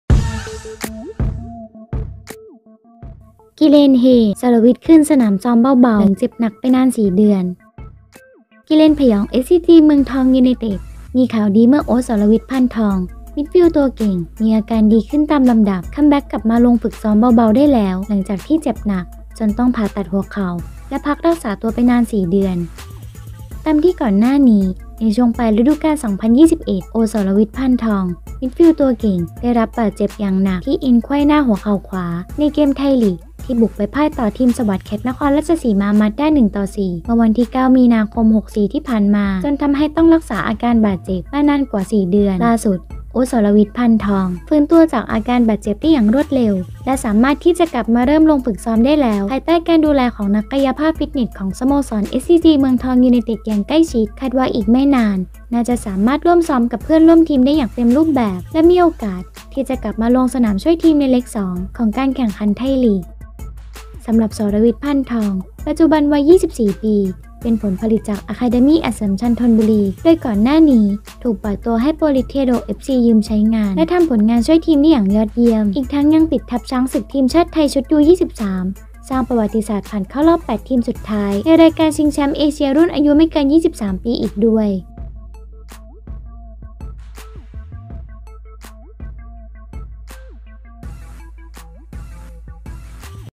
กิเลนเฮ ! “สรวิทย์” คืนสนามซ้อมเบาๆ หลังเจ็บหนักไปนาน 4 เดือน
"กิเลนผยอง" เอสซีจี เมืองทอง ยูไนเต็ด เฮ ! ข่าวดี ! "โอ๊ต" สรวิทย์ พานทอง  มิดฟิลด์ตัวเก่ง อาการดีขึ้นตามลำดับ คัมแบ็กกลับมาลงฝึกซ้อมแบบเบาๆได้แล้วหลังเจ็บหนัก จนต้องผ่าตัดเข่า และพักรักษาไปนาน 4 เดือน